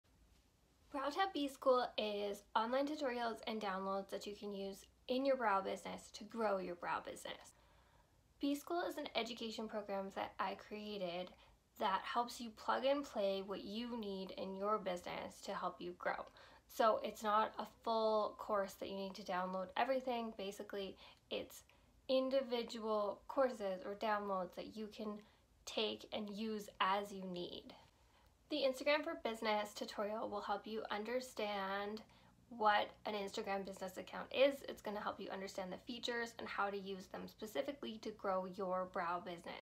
BrowTab B School- Online Education for Microblading Artists to grow their Brow Businesses
BrowTab B School is an Online Education Program that gives you the knowledge you need to help grow your Brow Business